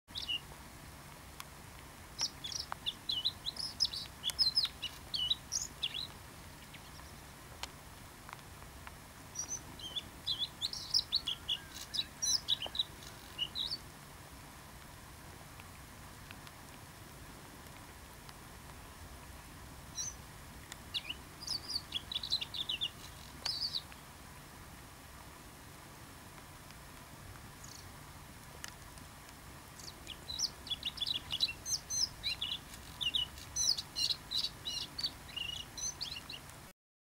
bluethroat
first winter male singing ria formosa natural park faro 30/10/2012